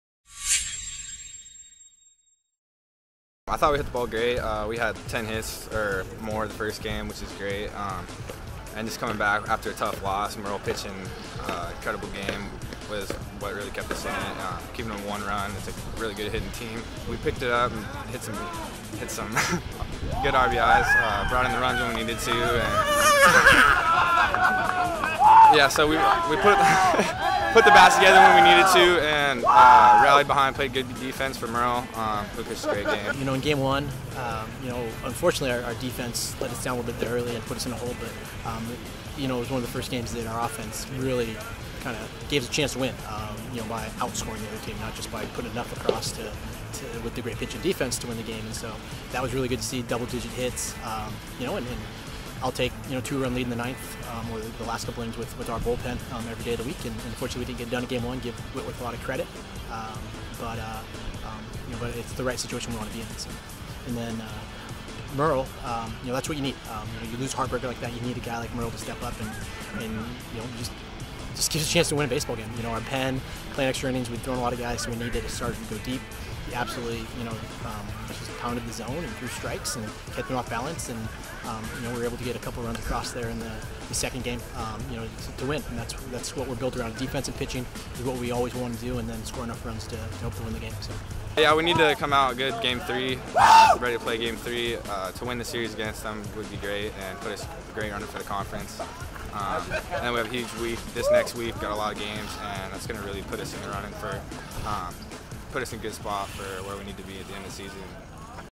Baseball vs. Whitworth (3.25.2017)
Highlights from the doubleheader split, and interviews with Matt Shelhamer and Jeff Halstead.